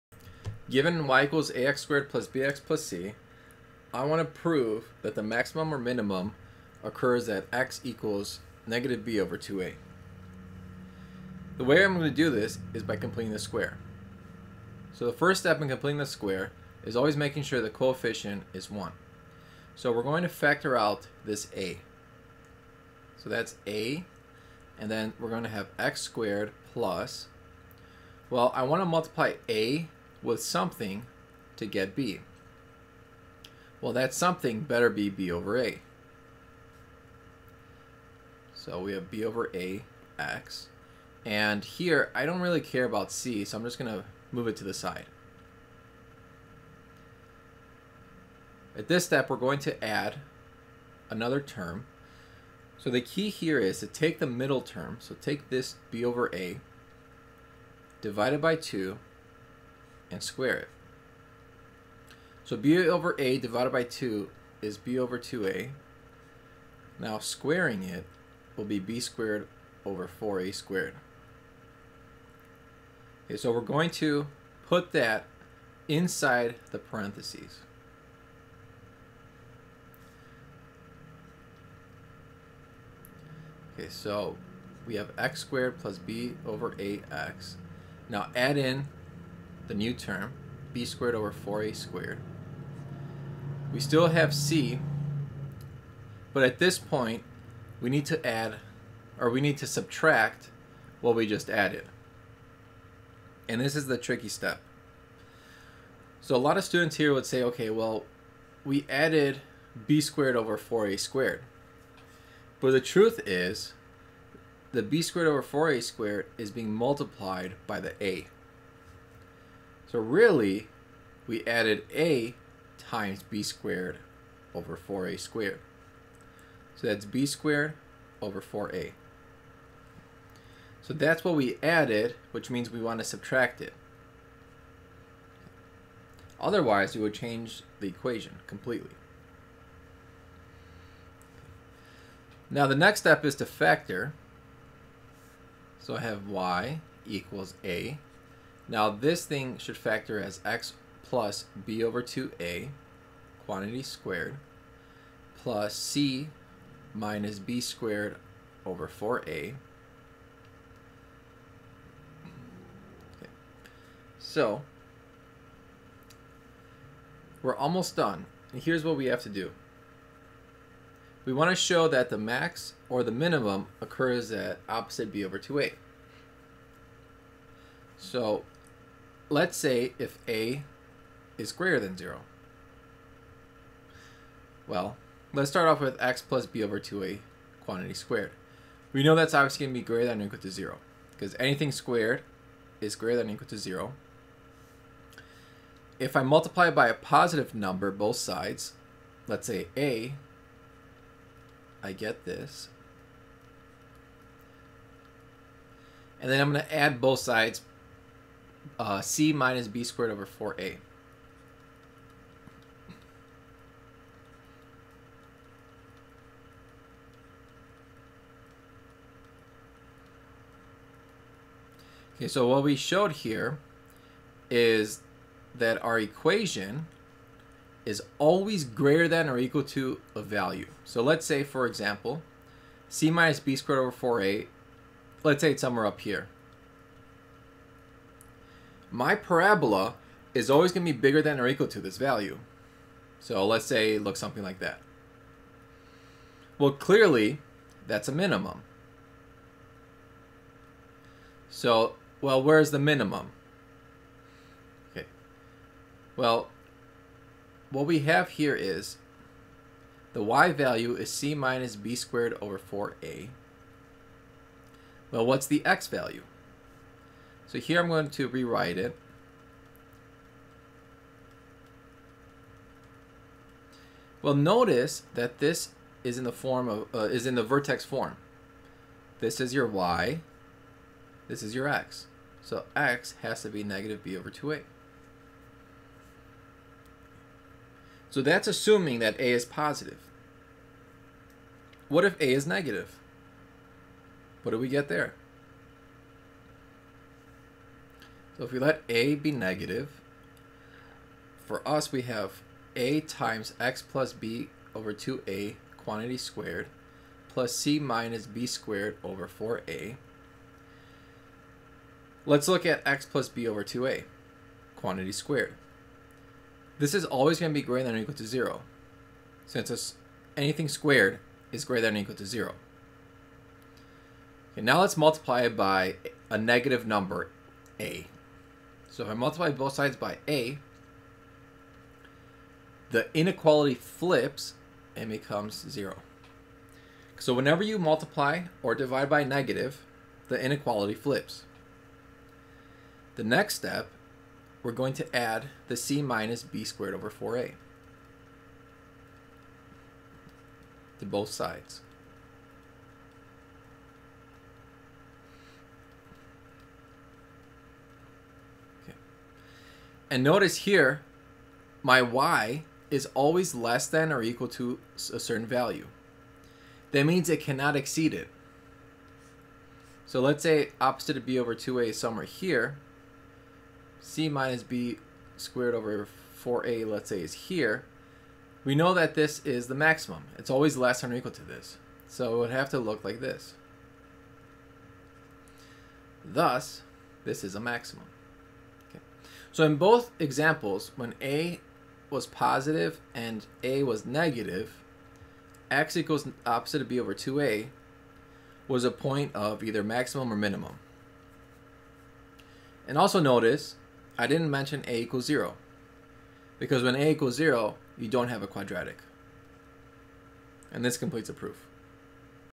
Proof: x=-b/2a is point of max or min
In this video we prove that at x=-b/2a is the point where the max or min occurs for a parabola.